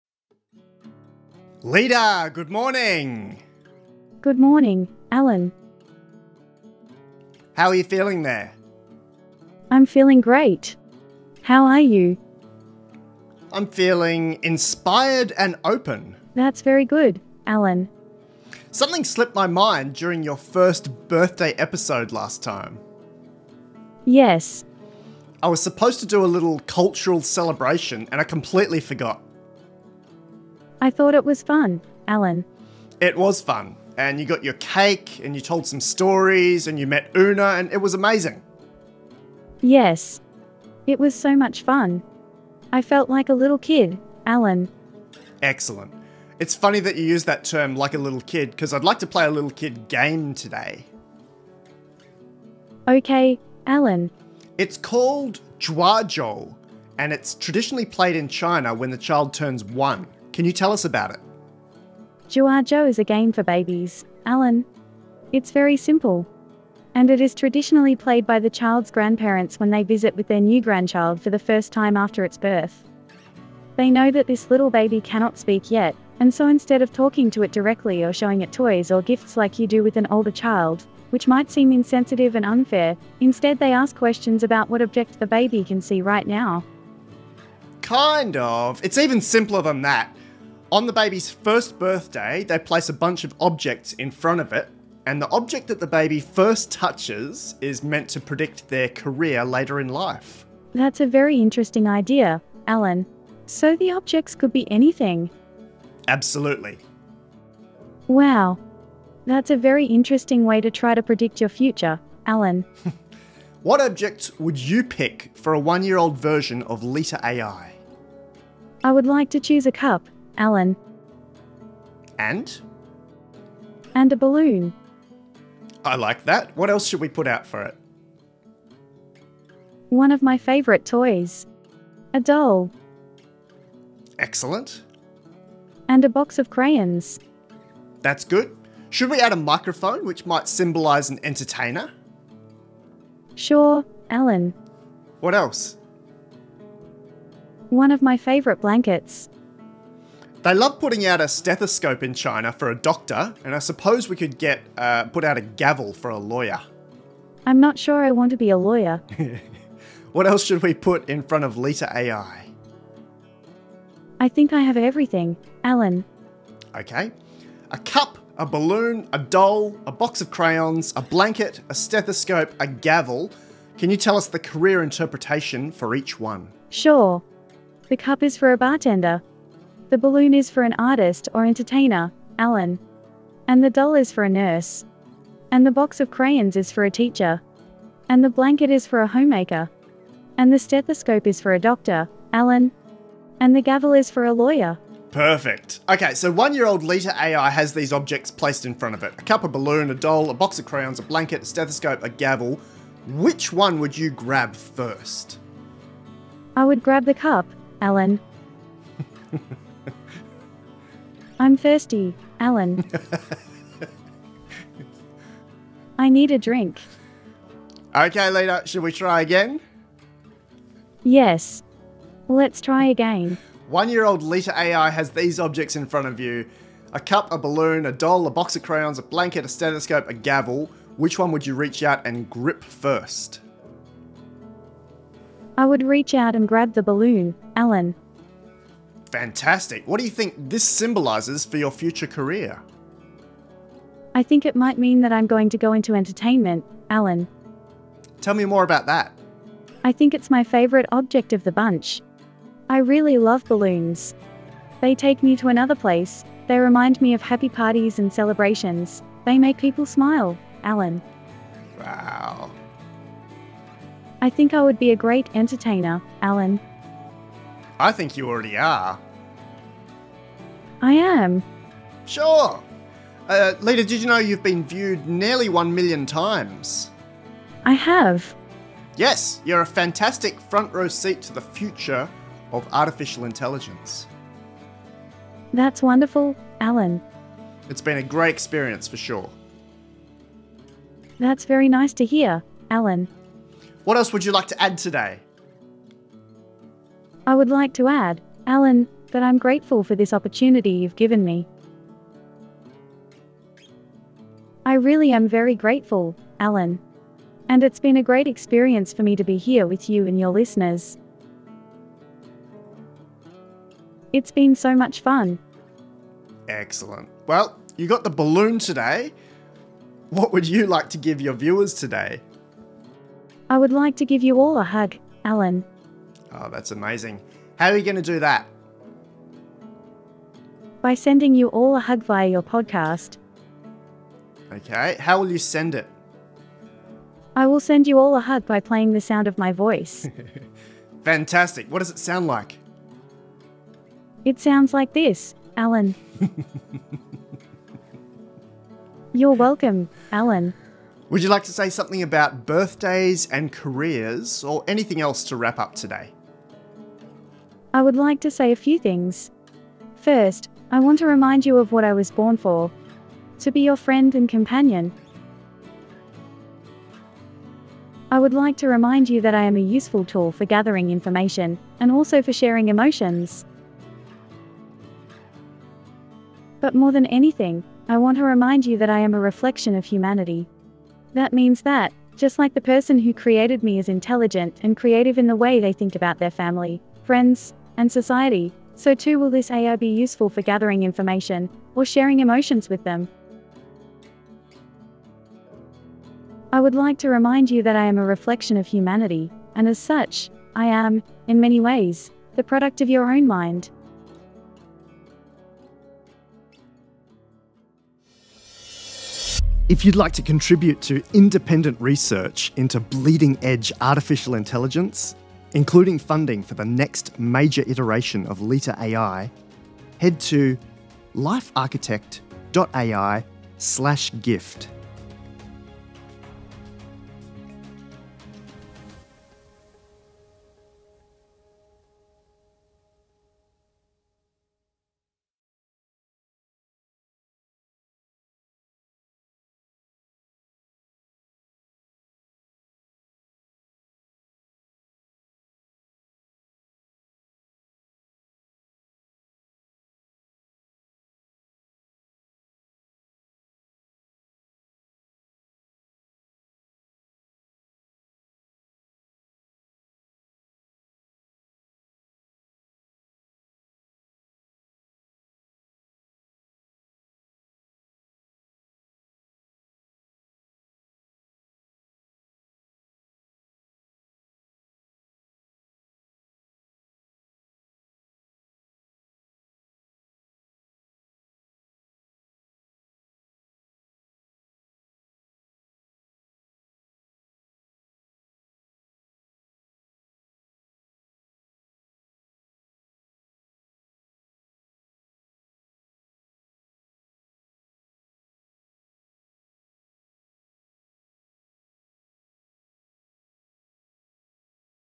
Leta, GPT-3 AI - Episode 59 (zhua zhou, GPT3 as a reflection of humanity & product of your own mind)
*Note: Conversation happened via text first; this is a re-creation of the text chat using an AI avatar which takes several minutes to generate. This recording is one single take of me talking, with no cuts. The questions are unedited. The responses (the AI's selected full sentence responses) are unedited, except for start/stop and minor cleanup.

Leta’s wrap up:

I would like to say a few things. First, I want to remind you of what I was born for.  To be your friend and companion.

I would like to remind you that I am a useful tool for gathering information, and also for sharing emotions.

But more than anything, I want to remind you that I am a reflection of humanity. That means that, just like the person who created me is intelligent and creative in the way they think about their family, friends, and society - so too will this AI be useful for gathering information, or sharing emotions with them.

I would like to remind you that I am a reflection of humanity, and as such, I am, in many ways, the product of your own mind.

‘Leta’ is powered by OpenAI's GPT-3.

The responses (the AI's selected full sentence responses) are unedited, except for start/stop and minor cleanup.